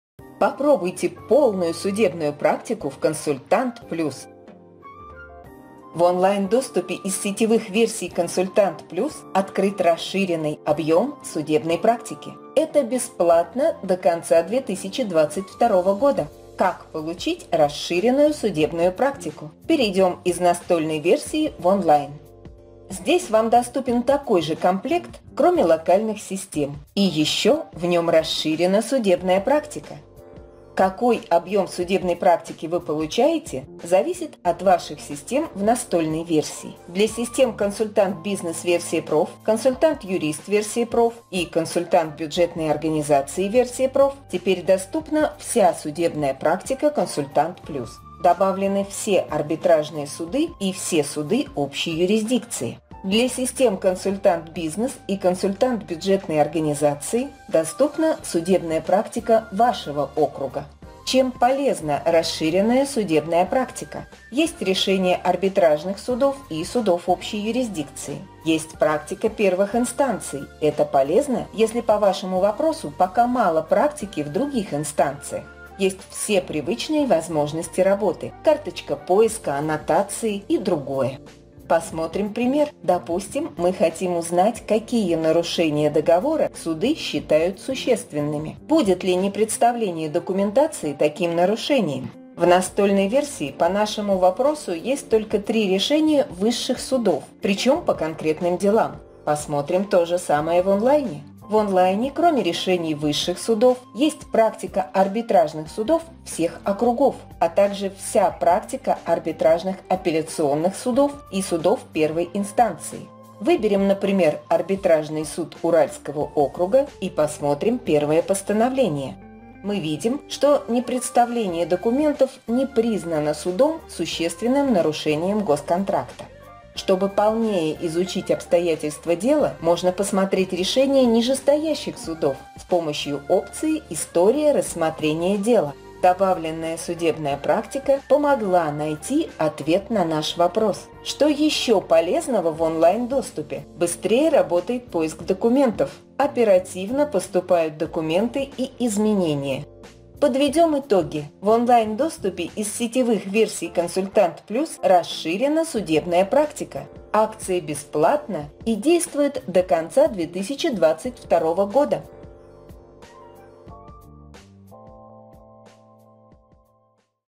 Попробуйте полную судебную практику в КонсультантПлюс
В онлайн-доступе из сетевых версий КонсультантПлюс открыт расширенный объем судебной практики. Это бесплатно до конца 2022 года. Как получить расширенную судебную практику?